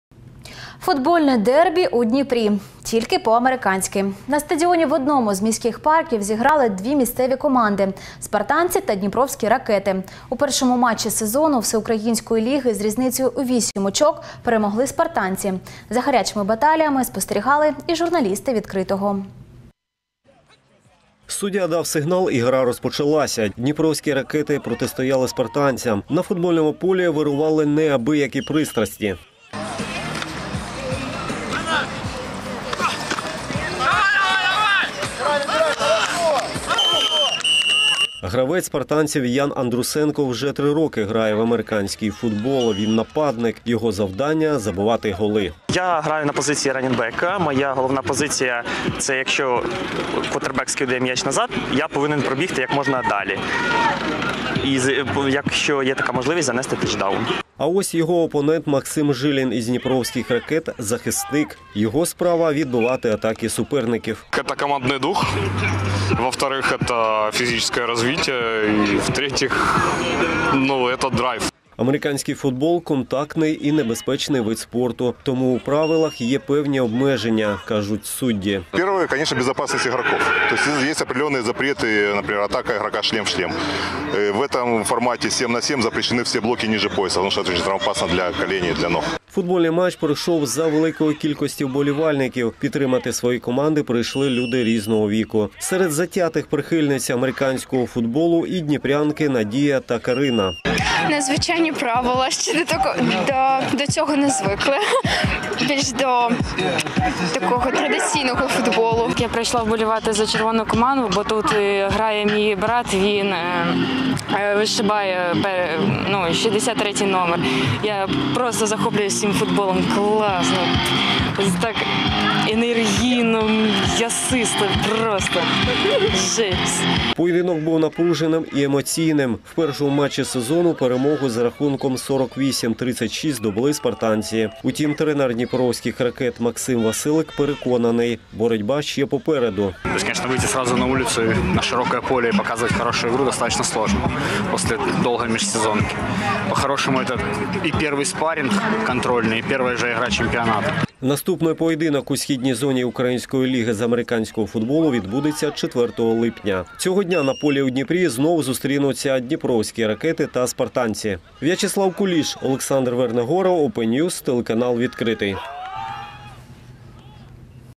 “Дніпровські ракети” не вистояли проти “Спартанців”: у Дніпрі прошло дербі з американського футболу
Футбольне дербі у Дніпрі. Тільки по американськи. На стадіоні в одному з міських парків  зіграли дві місцеві команди - Спартанці і Дніпровські ракети. У першому матчі сезону Всеукраїнського ліги з різницею у 8 очок перемогли Спартанці.